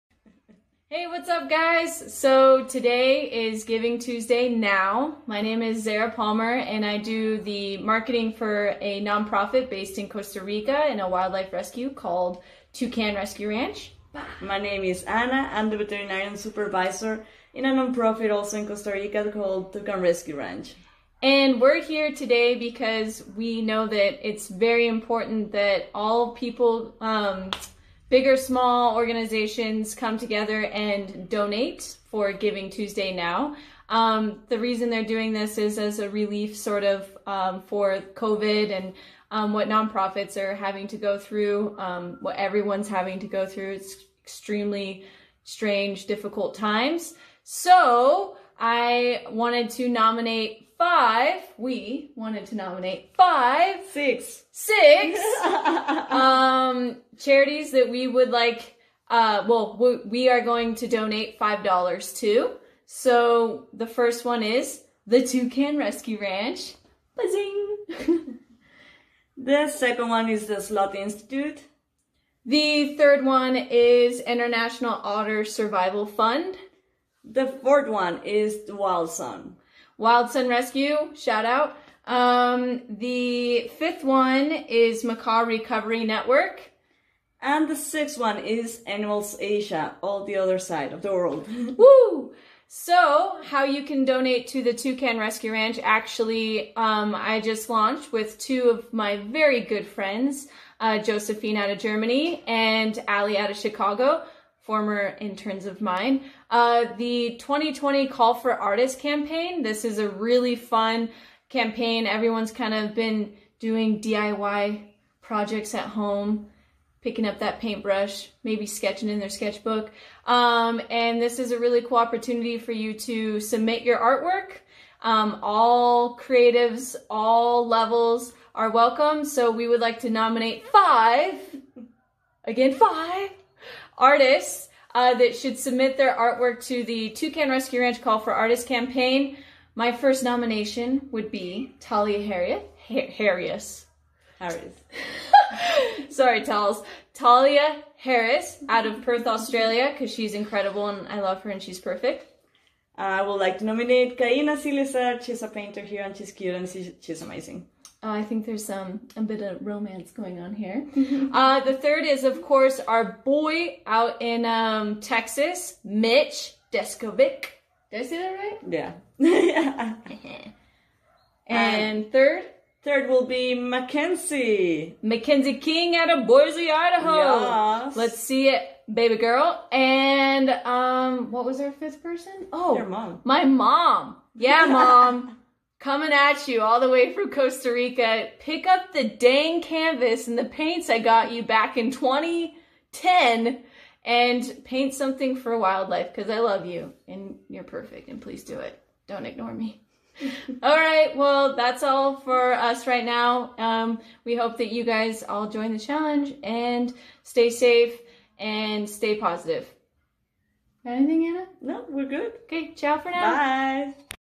Toucan Rescue Ranch GivingTuesdayNow | Zara Palmer & Ana María Villada Rosales
Art & Donor Involvement:
In 2019, Toucan Rescue Ranch launched its first-ever Call for Artists crowdfunding campaign. During that year's efforts, we raised $8,839 with 63 donors from all over the world for new medical equipment for its headquarters clinic! We overreached our goal by $6,272 and were able to purchase an anesthesia machine, operation lamp, IV Stand, and autoclave.
All of that equipment is in daily use to this day. With 41 submissions from our first campaign, we hope to have those people submit again and (perhaps even) MORE.

🌎 This year's campaign will act as a RELIEF FUND, which will help us maintain our daily animal-care operations. The money raised from this campaign will help us with our mission to feed, treat, rehabilitate, enrich, and release wildlife!

Artwork Submission Guidelines:
What to include with your submission:
Name of Artist
Age of Artist
Your Location
Title of Artwork
Medium Used (oils, acrylics, colored pencil, clay, etc.)
Recommended:
Your Social Media Handle(s)
The Inspiration Behind Your Submission
TRR will post submissions on our Facebook, Instagram, and Twitter.
Once posted, we will encourage people to VOTE (donate toward) their favorite submission.
The artwork with the most VOTES (donations) wins and will be used on our next CustomInk T-Shirt fundraising campaign.
Don't be discouraged, if your artwork does not win, TRR will surely make sure you are recognized with a "Recognition Post".
CALL FOR ARTISTS RULES:
You may only submit ONE piece of artwork.
The submission period 🕦 ENDS MAY 15 AT 11:59 P.M. COSTA RICAN TIME.
The artwork must follow the theme of 🐵"YOUR WISH FOR WILDLIFE"
A parent must submit your artwork if you are a minor with all the necessary information.
Only artwork submitted through EMAIL will be considered a part of this contest.
🚫 No photography as art submissions.
FOR DONORS: How do I Vote?
💸 $1.00 donated = 1 VOTE toward your artwork
To VOTE for a submission:
Make a "DEDICATION NOTE" when donating right here on MightyCause. A step-by-step video has been provided at the bottom of this page on how to do so.
The photo of each category that receives the MOST VOTES by the end of the voting period ⌛(May  30 at 8:00 p.m. Costa Rican Time) is the WINNER!